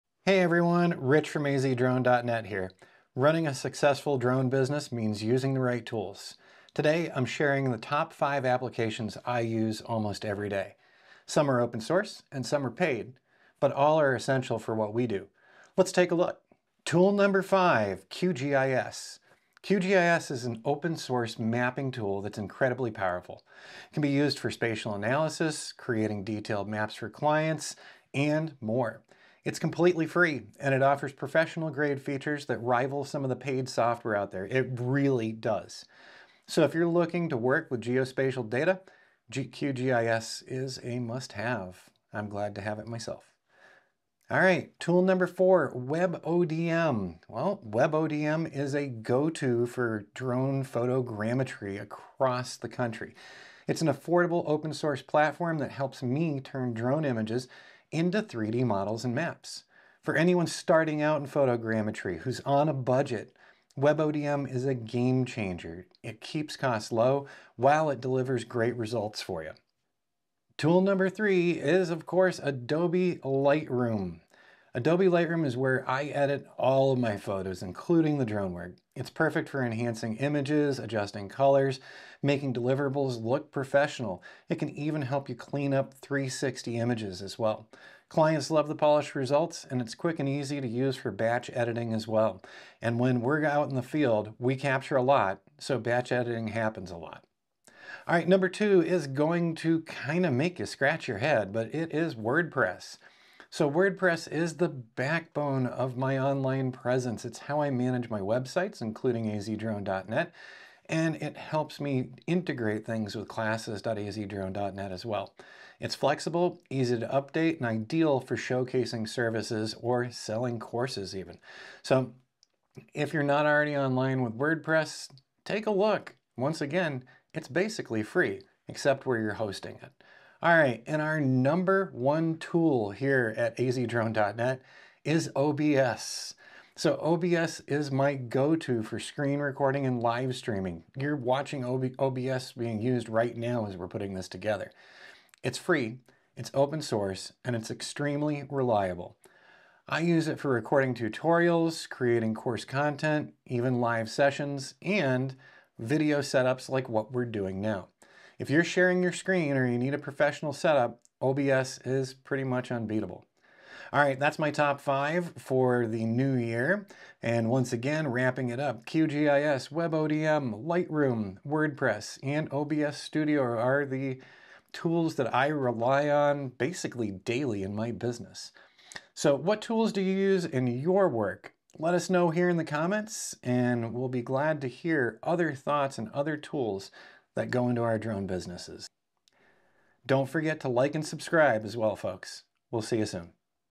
Top 5 Applications For Our Drone Business
Discover the top 5 applications I use daily in my drone business to create and deliver professional-quality client projects! These tools are essential for mapping, modeling, video production, and sharing final deliverables:

QGIS: For powerful geospatial analysis and mapping.
WebODM: To process drone imagery into stunning 2D maps and 3D models.
OBS Studio: For seamless screen recordings and live demonstrations.
WordPress: To build and manage custom client web pages.
Adobe Lightroom: For enhancing drone photos to perfection.

•Check out AZ Drone’s Complete Class Series Bundle - Save 30% For 18 Classes With Coupon “30PERCENT”